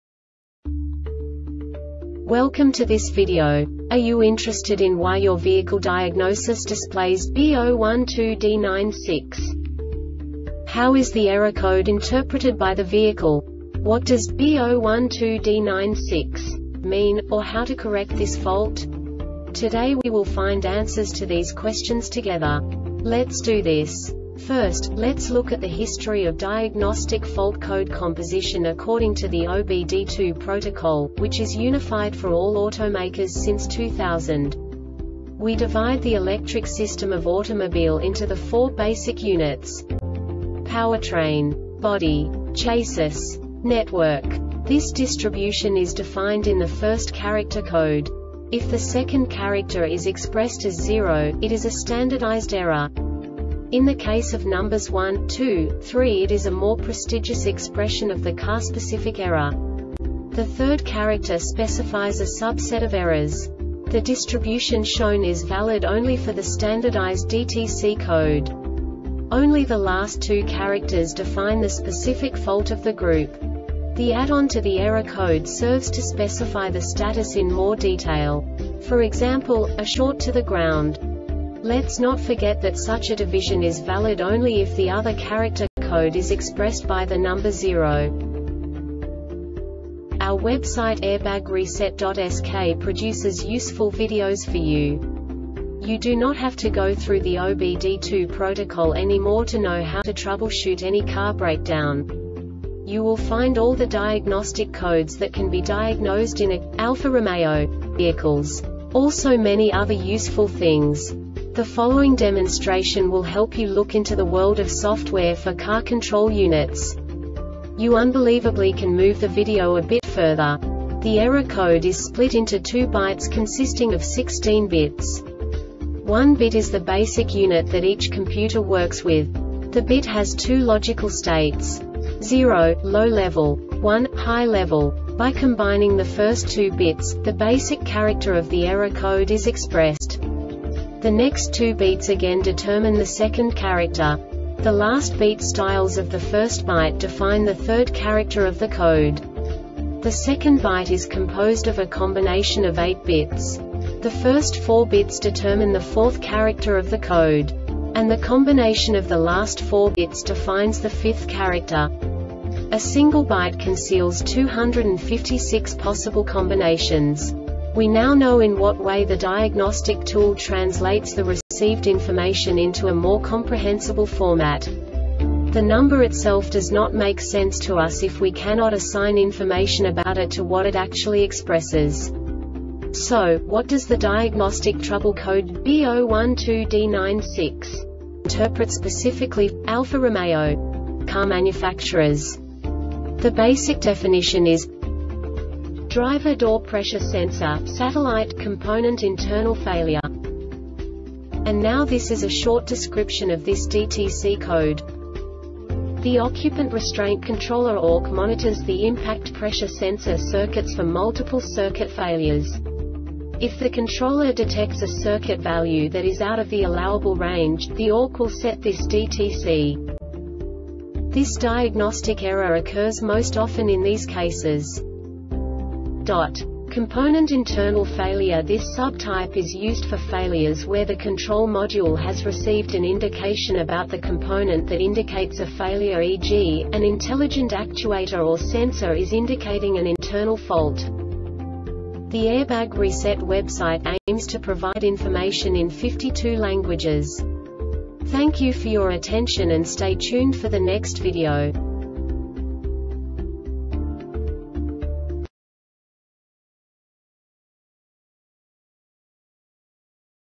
DTC Alfa-Romeo B012D-96 Short Explanation
Contents:
0:21 Basic DTC analysis according to OBD2 protocol standard.
1:48 Insight into programming
2:58 A diagnostically understandable description of the Diagnostic Trouble Code
3:05 Basic error definition

Make:
Alfa Romeo
Code:
B012D-96
Definition:
Driver Door Pressure Sensor (Satellite)-Component Internal Failure
Description:
The Occupant Restraint Controller (ORC) monitors the Impact or Pressure Sensor circuits for multiple circuit failures. If the controller detects a circuit value that is out of the allowable range, the ORC will set this DTC.